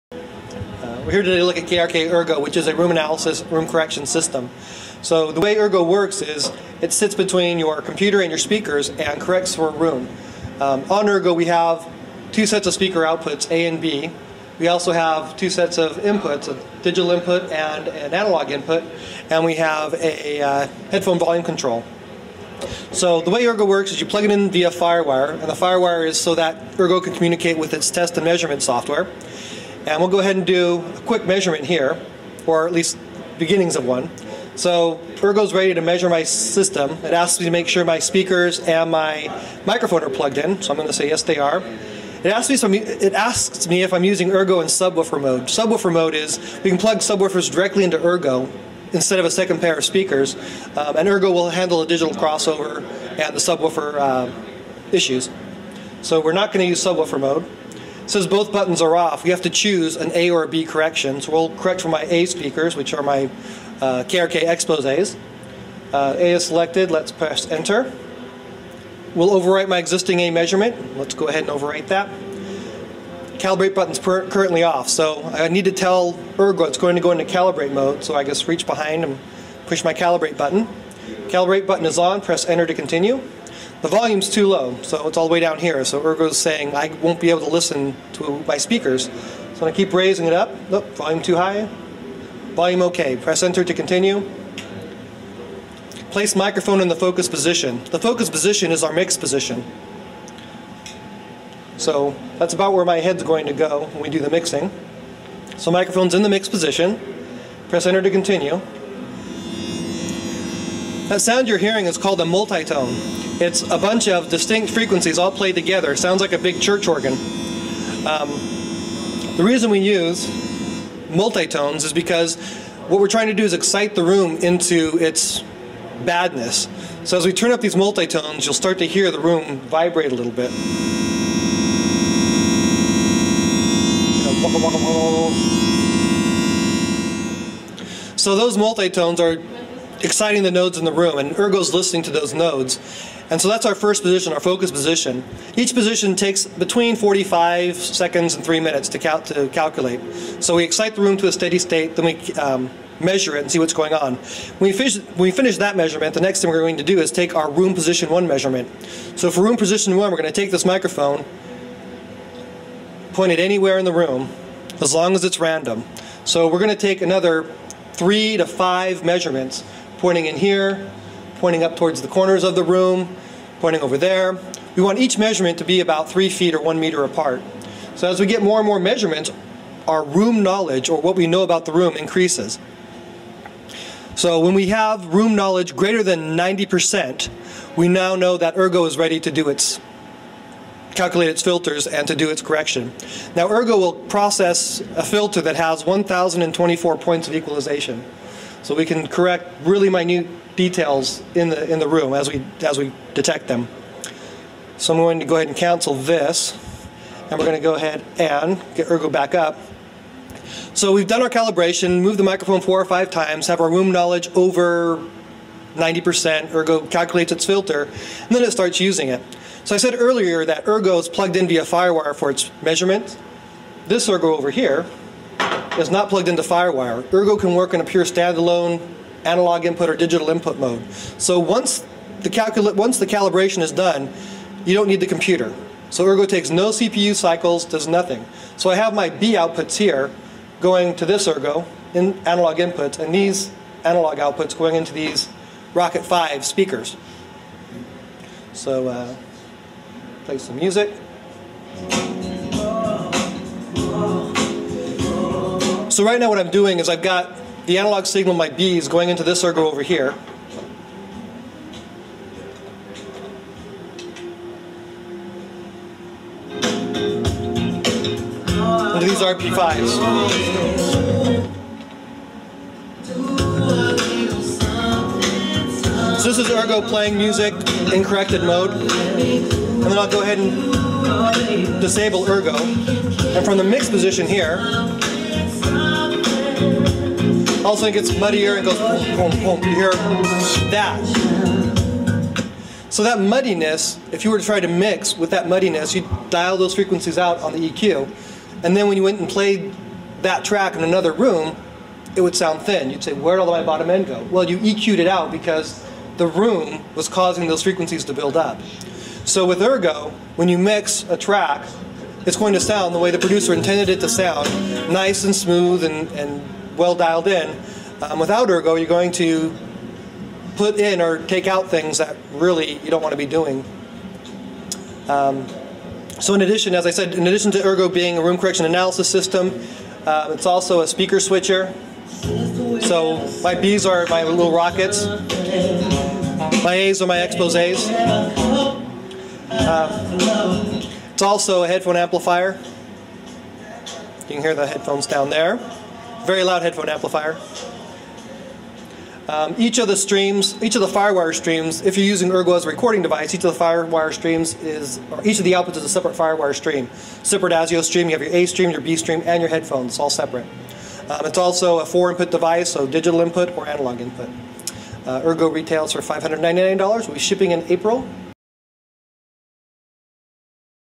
ERGO Room Correction Demo
A demo and fairly in-depth look at KRK's ERGO room correction analysis system.